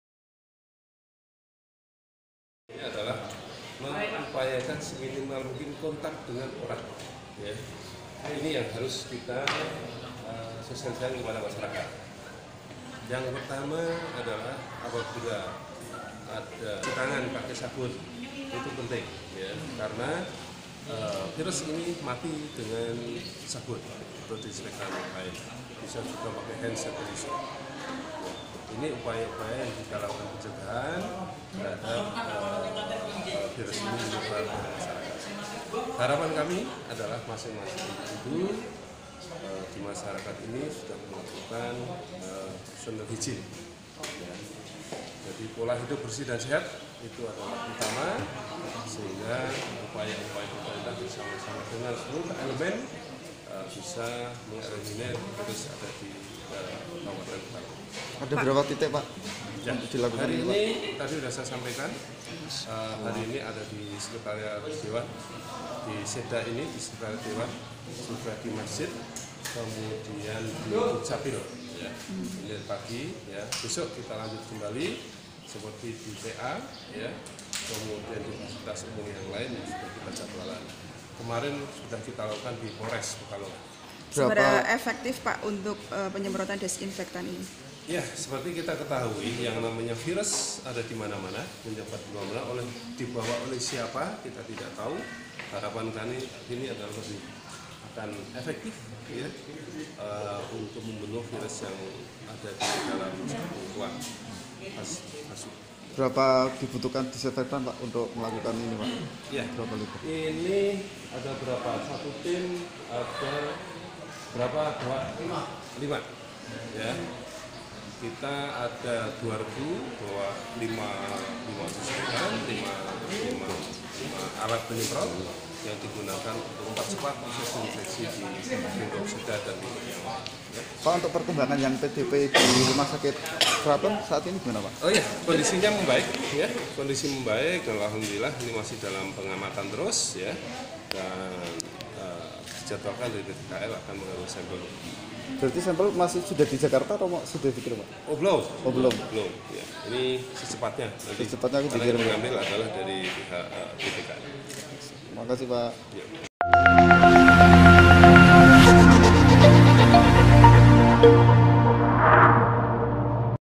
Pemkab Pekalongan Disinfektan Kantor Pemerintahan
KAJEN-Guna mengantisipasi penyebaran covid-19 Pemkab Pekalongan melakukan disinfektan di sejumlah kantor pemerintahan dan pelayanan kepada masyarakat seperti kantor Dukcapil pagi ini.

Bupati Pekalongan Asip Kholbihi melalui Kepala Dinas Kesehatan Kabupaten Pekalongan Setiawan Dwi Antoro mengatakan, kegiatan ini dilakukan pihaknya untuk mencegah terjadinya hal tidak diinginkan terkait covid-19